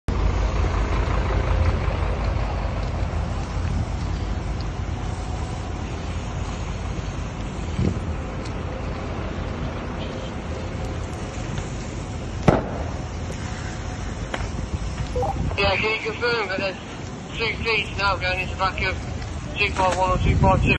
The fire at Kev's Coaches broke out at 10pm last night.
More than ten fire crews from both West Midlands Fire Service and Hereford and Worcester Fire and Rescue Service attended the massive blaze which broke out at Kev's Coaches, near Lydiate Ash in Bromsgrove.